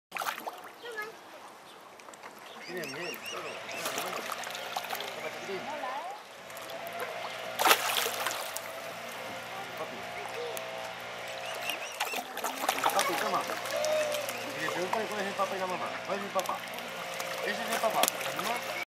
hungry fishes on the water surface 01
In a park, in lake, lots of hungry fish. Passersby throw them each a piece of bread, and they quickly eat.